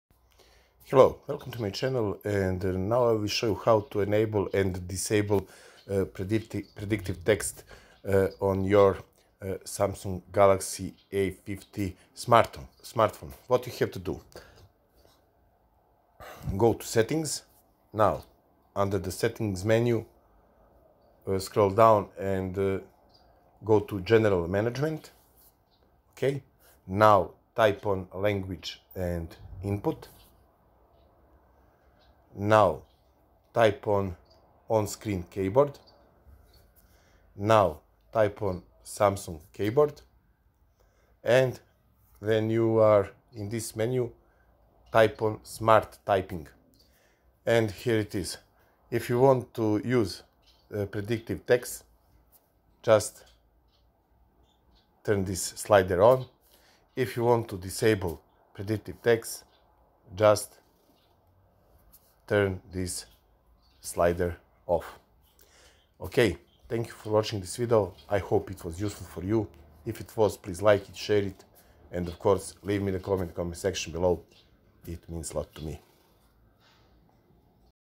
Samsung a50 how to disable/enable predictive text
How to turn on and off predictive text whyle typing on samsung galaxy a50